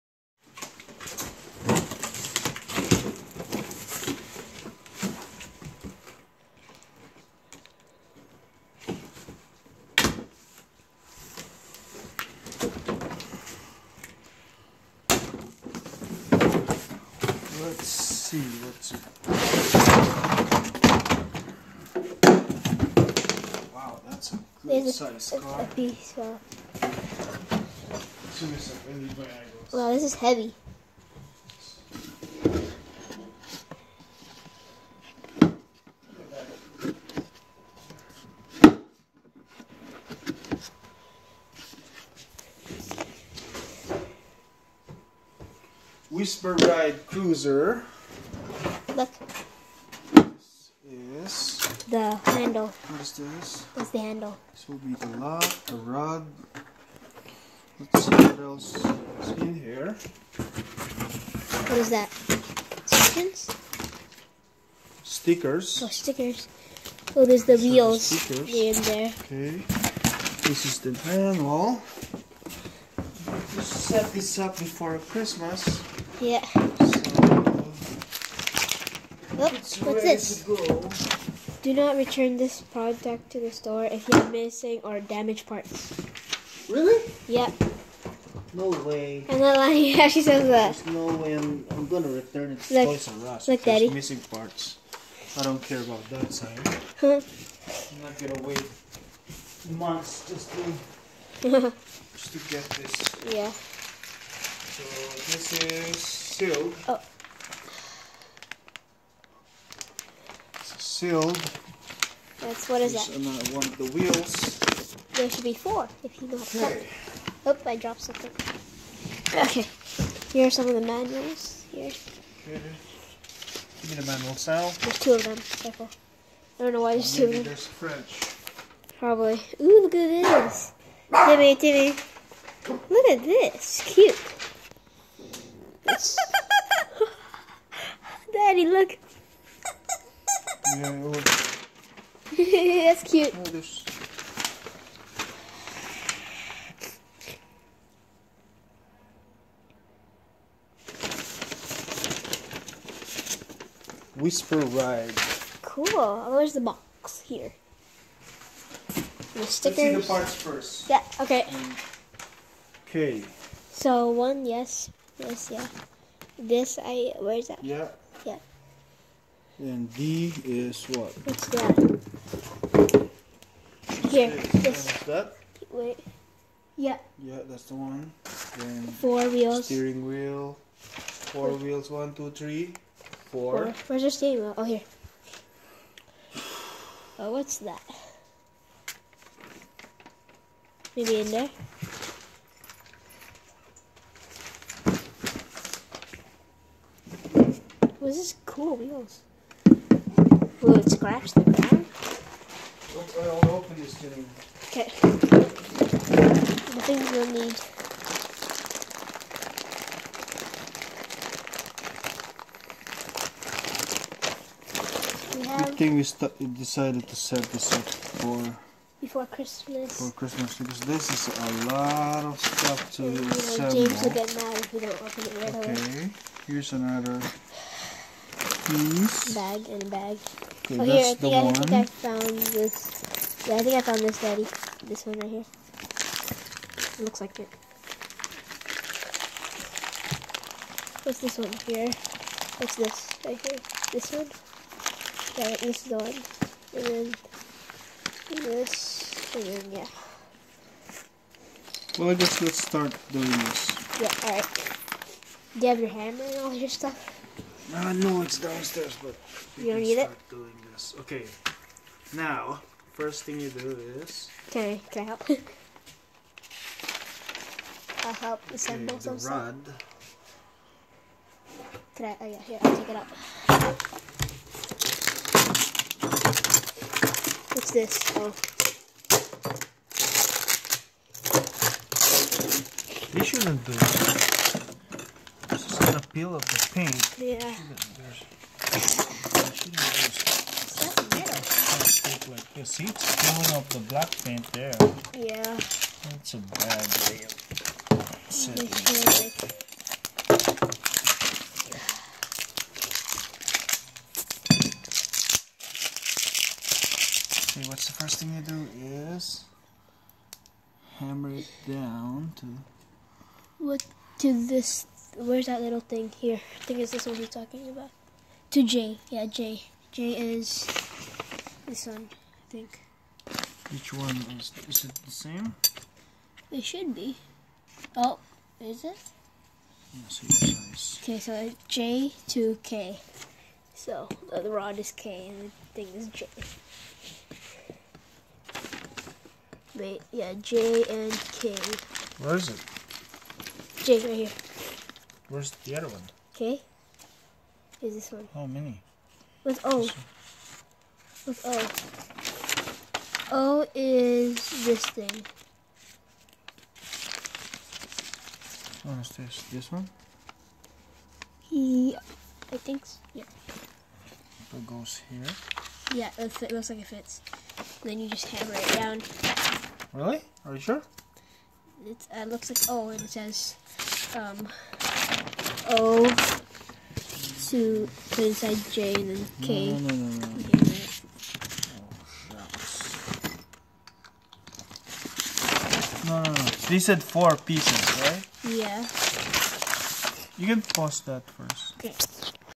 "Step2 Whisper Ride Cruiser" unboxing ...For Christmas Eve
Assembling this is NOT simple and quick, so good thing we did it before wrapping it.
Getting ready for Christmas Eve.